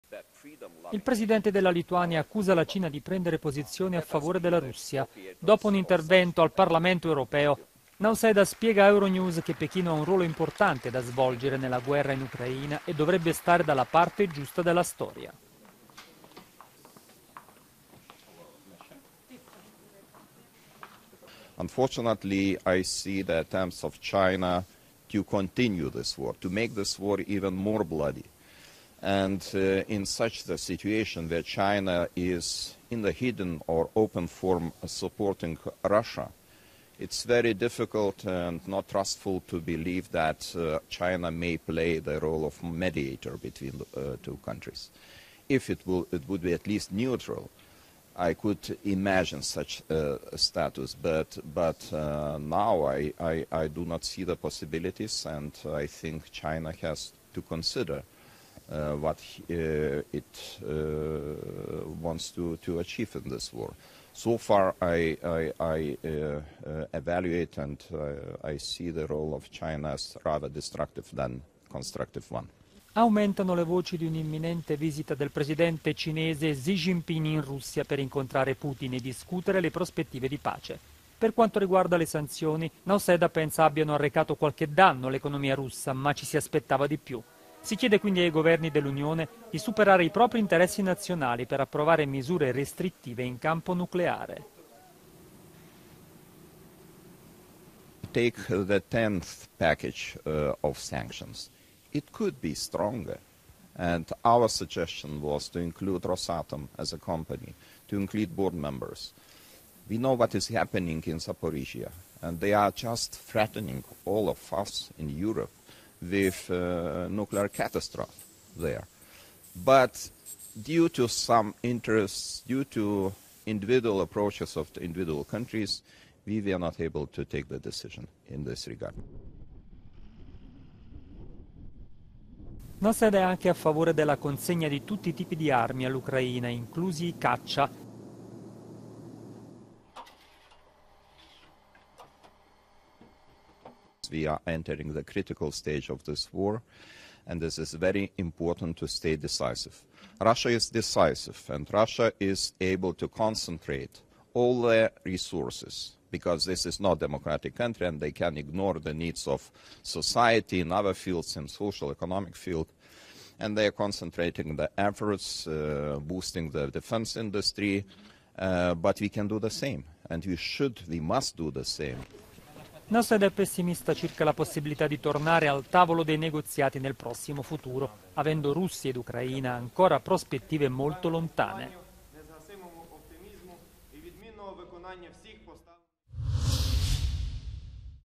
Lituania, il presidente accusa la Cina di prendere posizione a favore della Russia
Dopo un intervento al Parlamento europeo, Gitanas Nauseda ha spiegato a Euronews che Pechino ha un ruolo importante da svolgere nella guerra in Ucraina e dovrebbe stare dalla parte giusta della storia